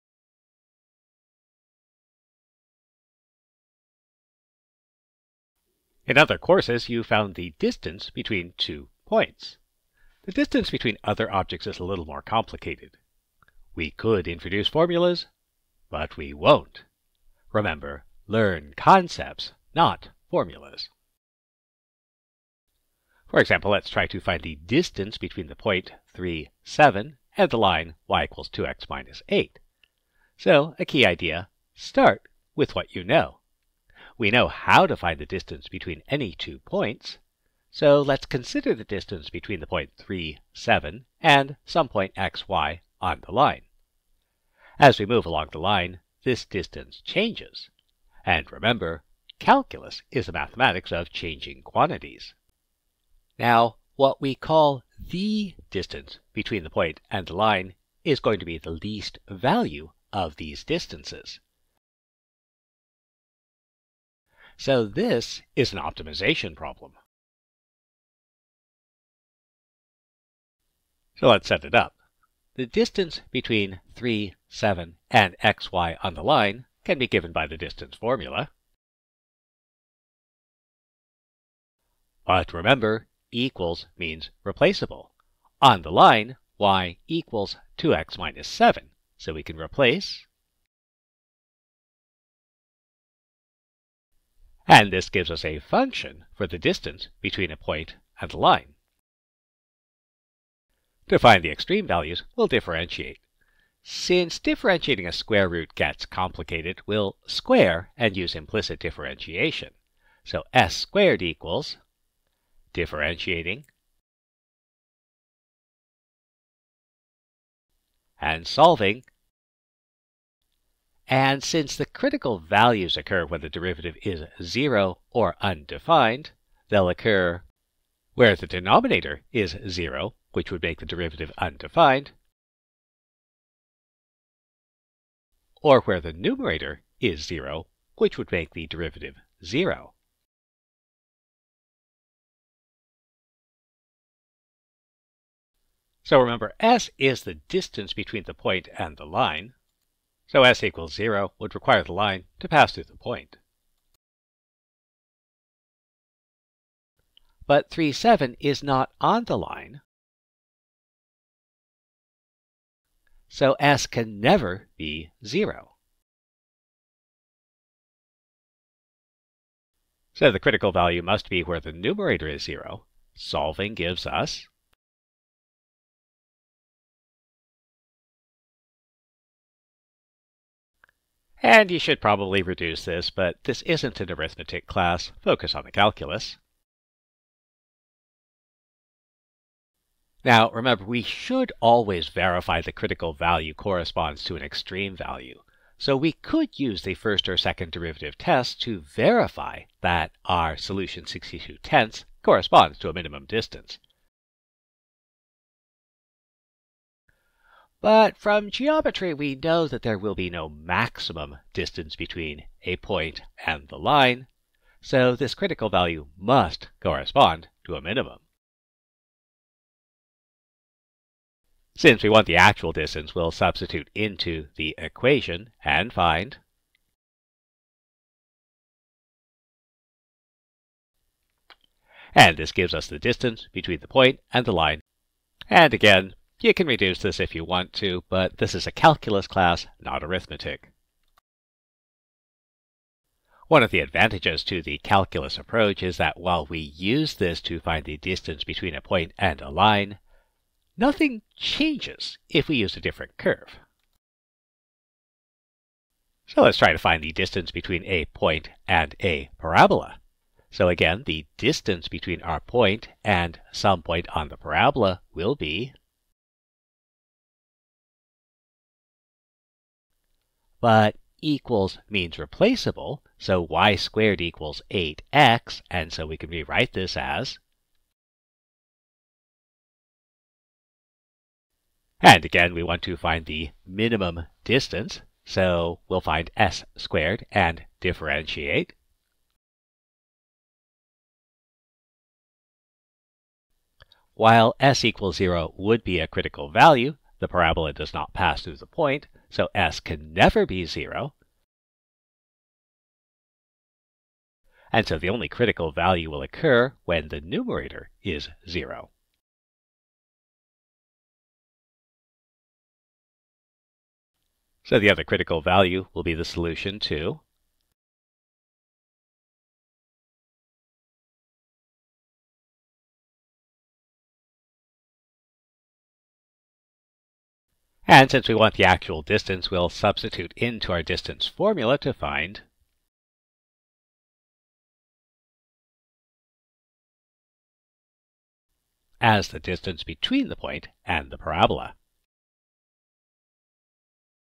Distances
Use calculus to find the distance between a point and a curve.